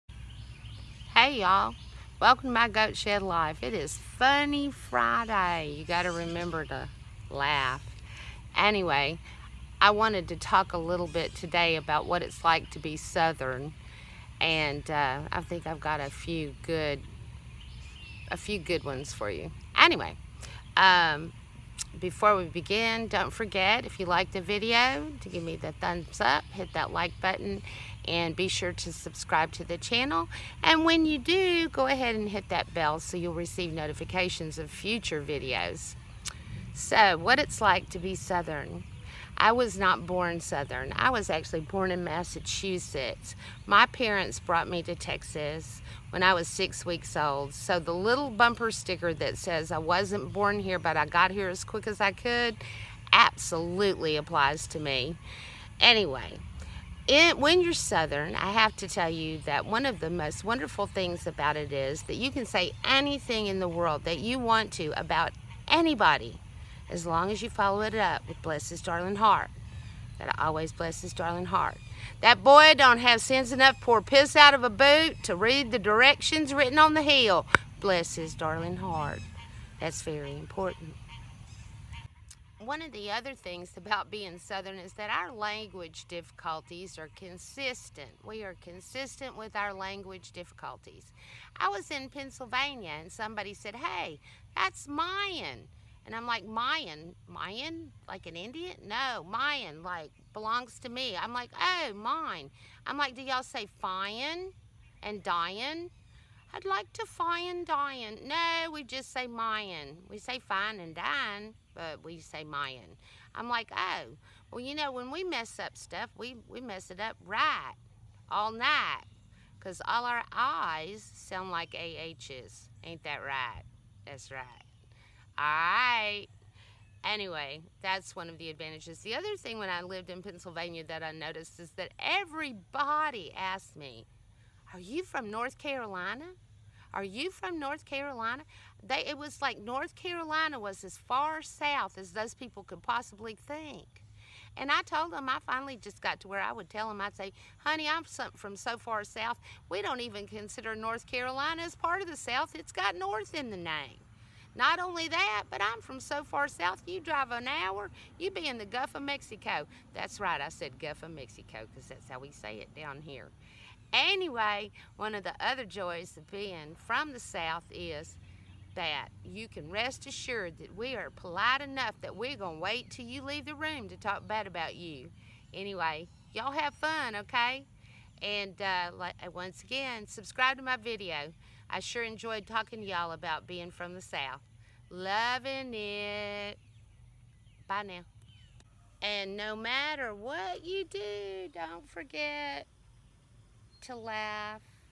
Funny Friday-Bein' Southern
This is a silly little video about being from the south.  I was born in Massachusetts, but my parents brought me here at six weeks old and I have live in the south most of my "lahf".  I love it, and love all the funny jokes about southerners as well.  I hope you enjoy!!!  Above all have fun, life is short.